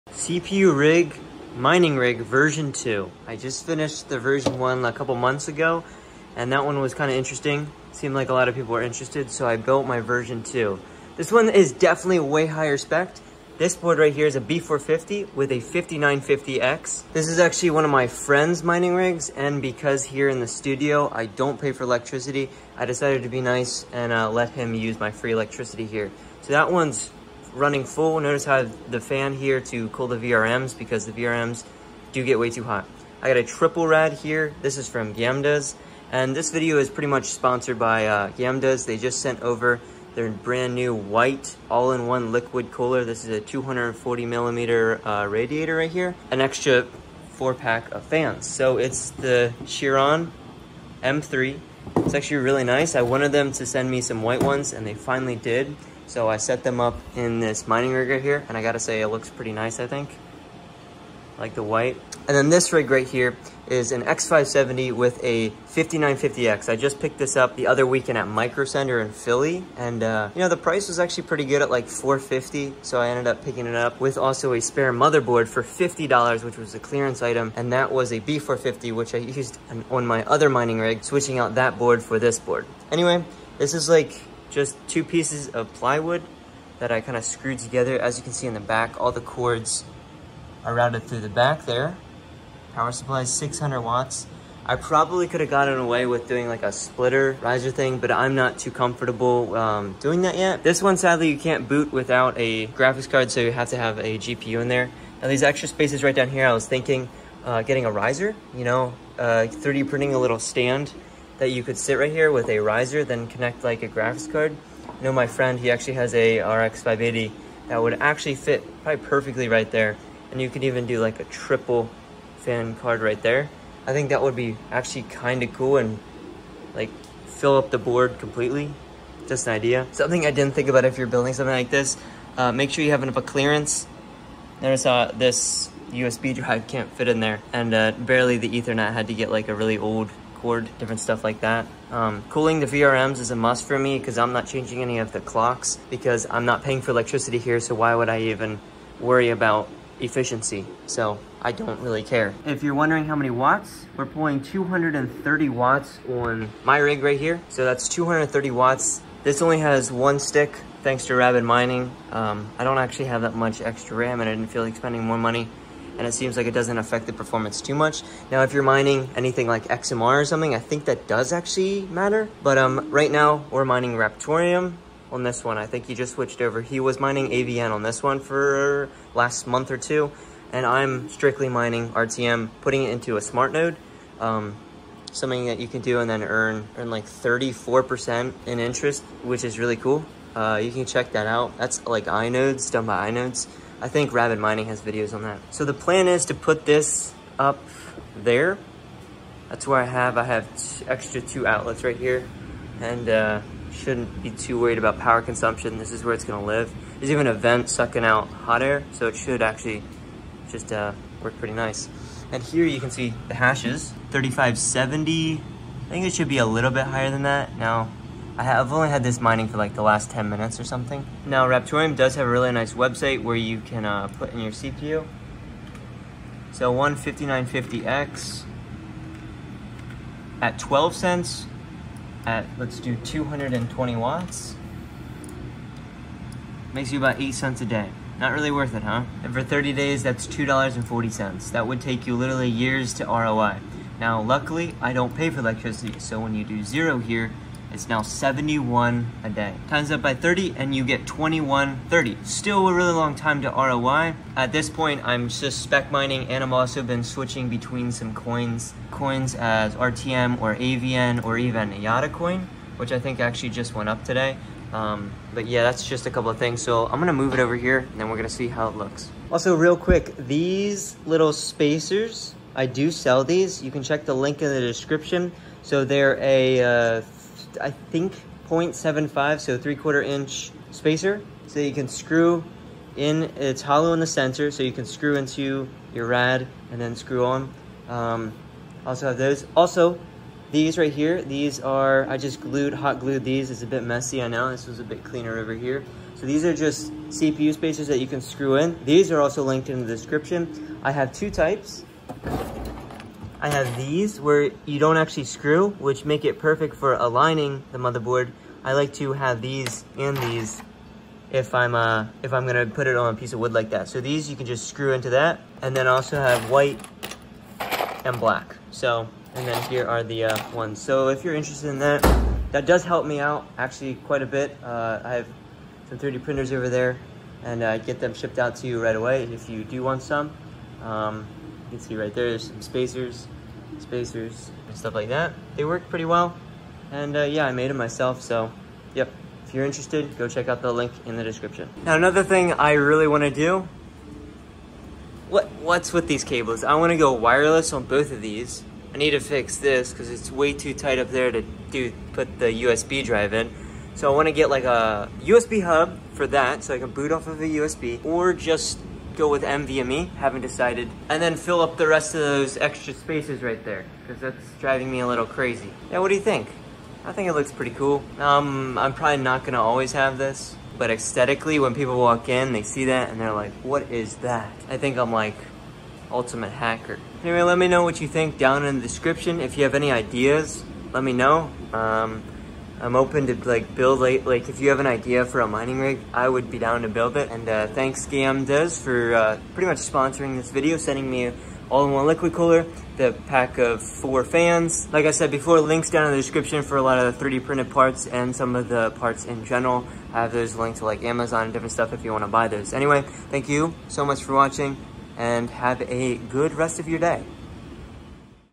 CPU Mining Rig V2 | RTM, AVN, YDA, & XMR Mining
GAMDIAS STORE➤

3D Printed Parts:
Motherboard Standoffs
Motherboard Standoffs Screwed
Radiator Standoffs
PSU Horizontal Bracket
PSU Vertical Bracket

Leave a comment about any other products that you might need.

So this is the second version of a CPU Mining Rig feature a 5950X and a 5900X. Be sure to checkout V1 with intel CPUs.

RYZEN 9 5950X
RYZEN 9 5900X

ASUS B450
MSI X570

Timeline Guide:
0:00 Setup
3:30 Hashrate
4:29 Rad Mounting Brackets
5:41 Conclusion